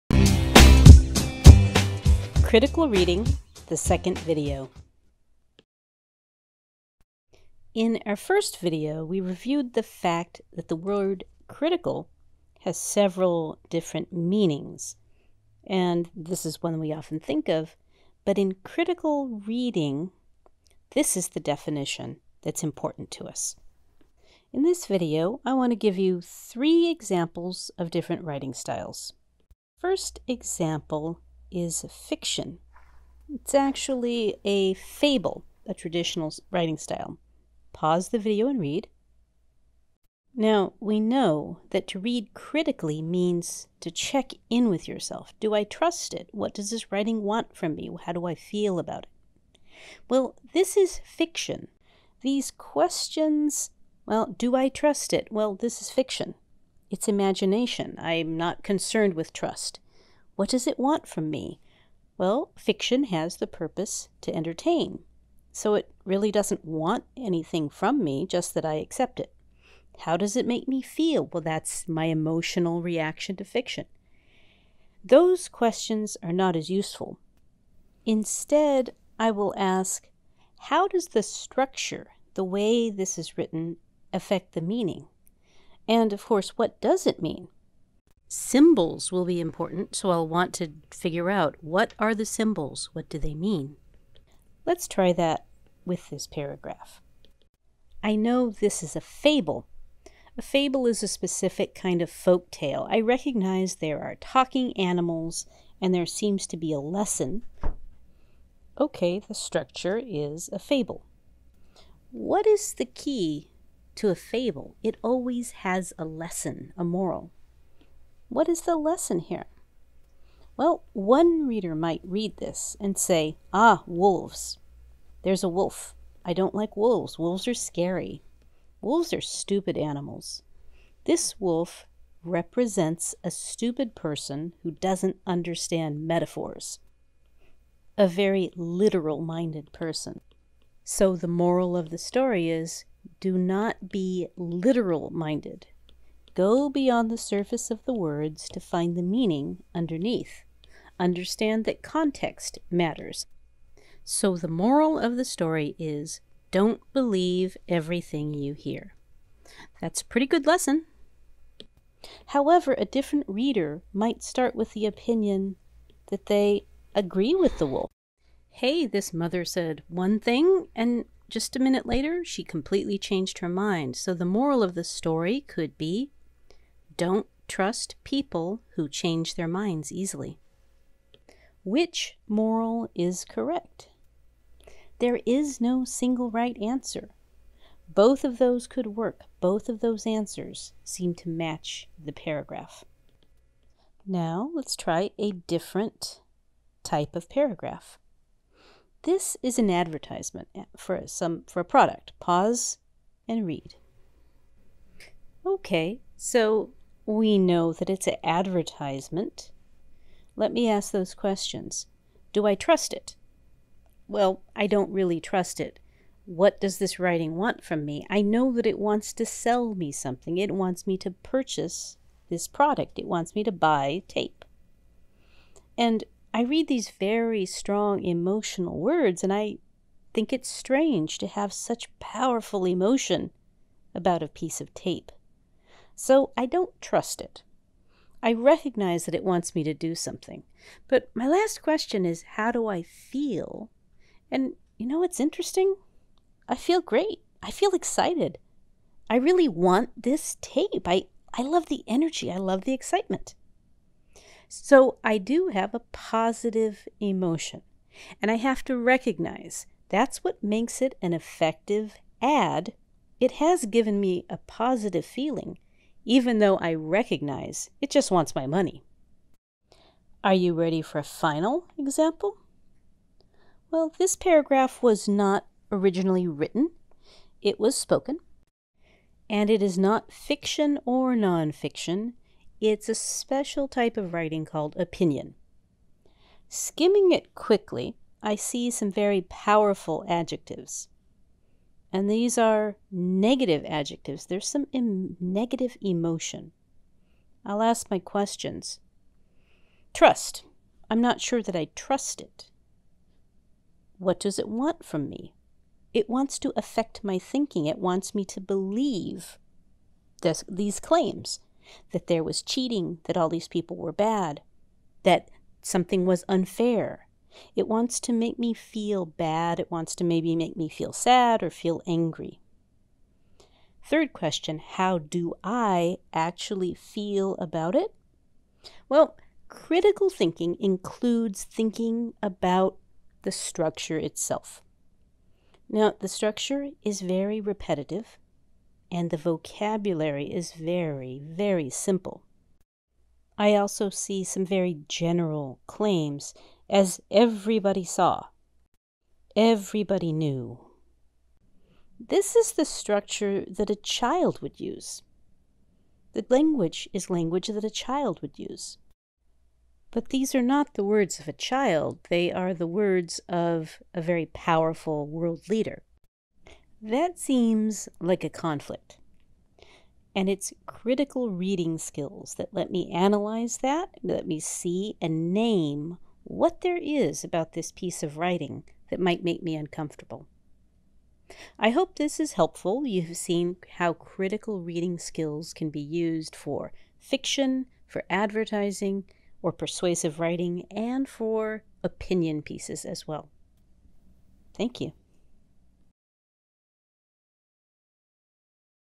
Critical Reading 2 of 2
A video to help students succeed in the Signature Assignment for ESOL 260. Examples of the reading skill Critical Reading are given for fiction, advertising, and opinion (persuasive) writing.